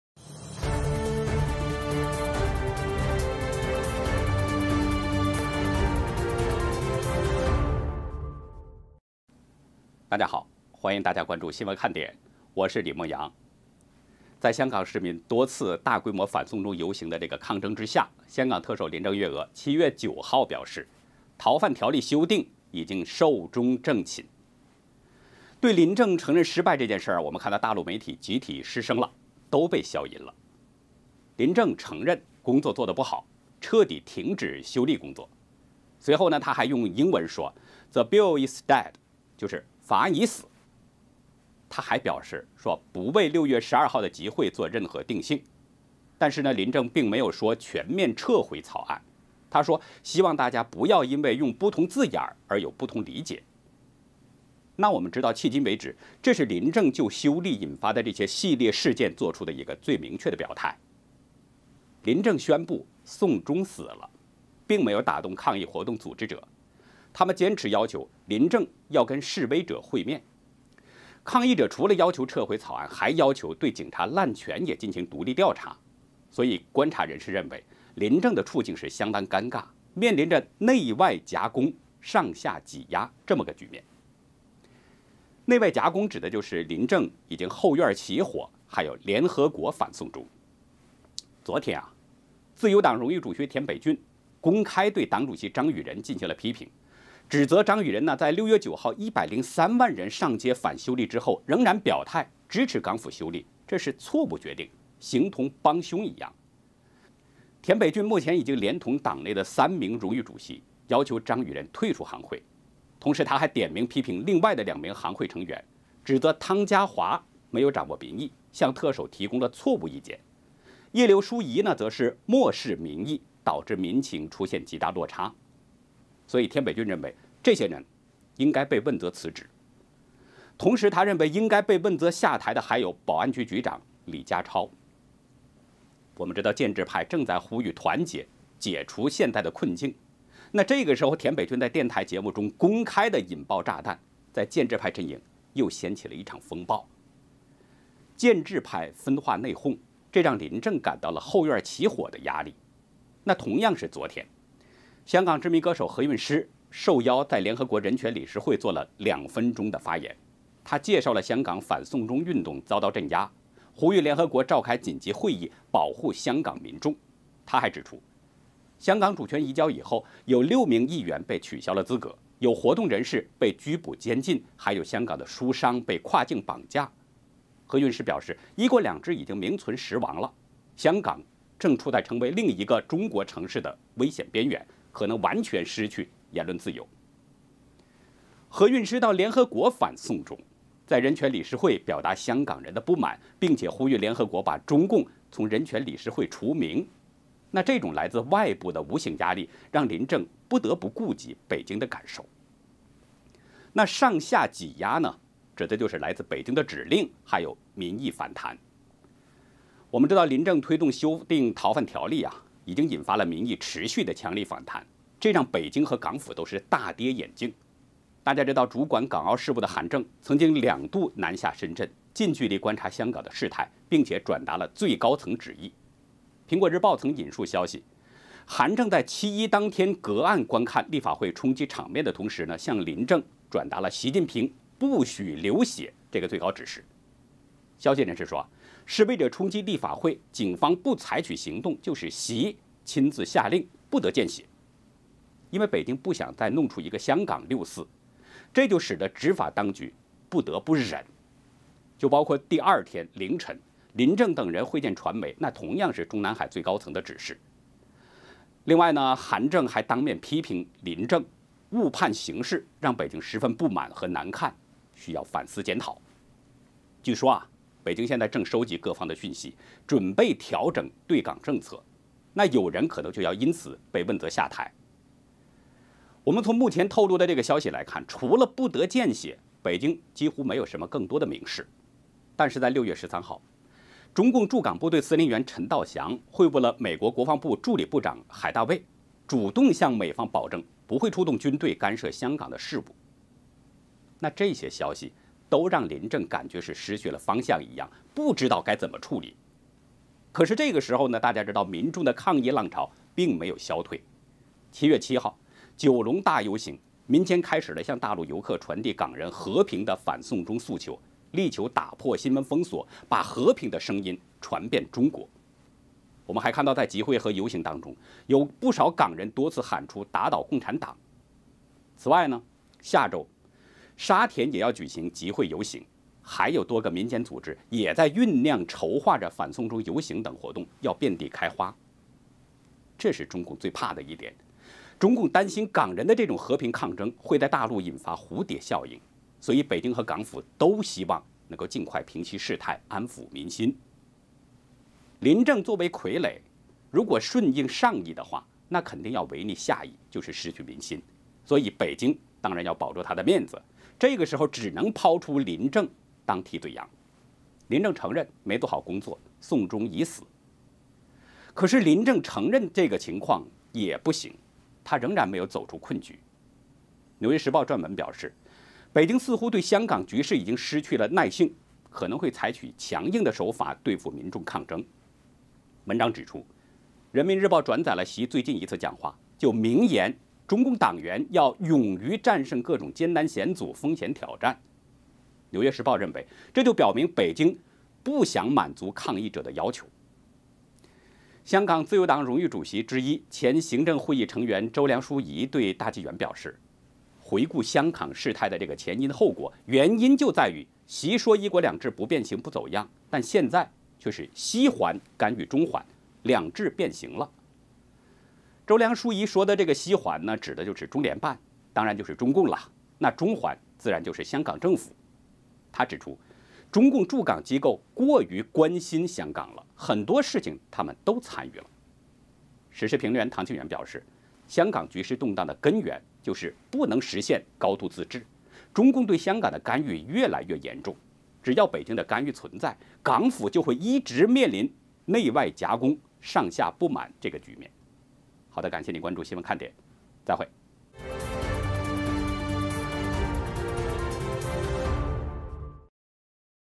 林郑称「送中」失败 走出困局？【中国禁闻】
【新唐人北京时间2019年07月10日讯】【新闻看点】林郑称「送中」失败  走出困局？（2019/07/09）（总第369期）

大家好，欢迎大家关注新闻看点，我是李沐阳。

在香港市民多次大规模反送中游行的抗争之下，香港特首林郑月娥今天（7月9日）表示，逃犯条例修订已经「寿终正寝」。对林郑承认失败这件事，大陆媒体集体「被消音」了。

林郑承认工作做得不好，「彻底停止修例工作」。她随后用英文说「The bill is dead」（法案已死）。她还表示，不为6月12日集会做任何定性。但她并没说「全面撤回」草案，她说「希望大家不要因为用不同字眼而有不同理解」。迄今为止，这是林郑就修例引发的系列事件做出的最明确表态。

林郑宣布「送中」死了，并没有打动抗议活动组织者，他们坚持要求林郑与示威者会面。抗议者除了要求撤回草案，还要求对警察滥权进行独立调查。观察人士认为，林郑的处境相当尴尬，面临着「内外夹攻」和「上下挤压」。

「内外夹攻」指的是林郑已经后院起火和联合国反送中。

昨天，自由党荣誉主席田北俊公开对党主席张宇人进行了批评。指责他在6月9日103万人上街反修例后，仍然表态支持港府修例是「错误决定」，形同「帮凶」。他已经联同党内的三名荣誉主席要求张宇人退出行会。

田北俊还点名批评另外两名行会成员，指责汤家骅没有掌握民意，向特首提供错误意见，叶刘淑仪漠视民意，导致民情出现极大落差。田北俊认为他们应该问责辞职，同时应该被问责下台的还有保安局局长李家超。

建制派正在呼吁团结解困之际，田北俊在电台节目中却公开「引爆炸弹」，在建制派阵营又掀起了一场风暴。建制派分化内讧，让林郑感到了后院起火的压力。

同样是昨天，香港知名歌手何韵诗受邀，在联合国人权理事会做了2分钟的发言。她介绍了香港反送中运动遭到镇压，呼吁联合国召开紧急会议保护香港民众。

他还指出，香港主权移交后，有6名议员被取消资格，有活动人士被拘捕监禁，还有香港书商被跨境绑架。何韵诗表示，一国两制名存实亡，香港正处在成为另一个中国城市的危险边缘，可能完全失去言论自由。

何韵诗到联合国「反送中」，在人权理事会表达香港人的不满，并且呼吁联合国把中共从人权理事会除名。这种来自外部的无形压力，让林郑不得不顾及北京的感受。

「上下挤压」，指的是来自北京的指令和民意的反弹。

林郑推动修订逃犯条例，引发民意持续强力反弹，让北京和港府都是大跌眼镜。为了主管港澳事务的韩正曾两度南下深圳，近距离观察香港事态，并且转达最高层的旨意。苹果日报曾引述消息，韩正在七一当天隔岸观看立法会冲击场面的同时，向林郑转达了习近平「不许流血」的最高指示。

消息人士表示，示威者冲击立法会，警方不采取行动，就是习亲自下令「不得见血」。北京不想弄出个香港六四，使得执法当局「不得不忍」。包括次日凌晨林郑等人会见传媒，同样是中南海最高层的指示。

韩正曾当面批评林郑误判形势，让北京十分不满和难看，需要反思检讨。据称北京正收集各方讯息，准备调整对港政策，有人可能会被问责下台。

从透露的消息来看，除了「不得见血」，北京没有更多明示。但在6.12事件第二天，中共驻港部队司令员陈道祥会晤美国防部助理部长海戴维（David Helvey），主动向美方保证，不会出动军队干涉香港事务。这些消息，让林郑似乎失去了方向，不知该如何处理。

而此时，民众的抗议浪潮并未消退。7月7日九龙大游行，民间开始向大陆游客传递港人和平的反送中要求。力求打破新闻封锁，把和平的声音传遍中国。在集会和游行中，港人多次喊出「打倒共产党」。此外，下周在沙田也将举行集会游行，还有多个民间组织在酝酿筹划反送中游行等活动，遍地开花。

这是中共最怕的一点，担心港人和平抗争在大陆引发蝴蝶效应。所以北京和港府都希望尽快平息事态，安抚民心。

林郑作为傀儡，如果顺应「上意」，可能就要违逆「下意」，失去民心。而北京当然要保住面子，所以只能抛出林郑当「替罪羊」。林郑承认没做好工作，送中已死。

但即使如此，林郑仍然没有走出困局。纽约时报撰文表示，北京似乎对香港局势「失去耐性」，可能会采用强硬手法对付民众抗争。文章指出，人民日报转载了习最近一次讲话，明言中共党员要「用于战胜各种艰难险阻、风险挑战」。文章认为，这表明北京无疑满足抗议者的要求。

香港自由党荣誉主席之一、前行政会议成员周梁淑怡对大纪元表示，回顾香港事态的前因后果，原因就在于习说一国两制「不变形、不走样」。但现在却是「西环干预中环，让两制变形了」。周梁淑怡说的西环指的是中联办，也就是中共。中环就是香港政府。她指出，中共「驻港机构过于关心香港了，很多事情他们都参与了」。

时事评论员唐靖远表示，香港局势动荡的根源，就是不能实现「高度自治」，中共对香港的干预越来越严重。只要北京的干预存在，港府就一直会面临内外夹攻、上下不满的局面。

好的，感谢您关注新闻看点，别忘了转发点赞，再会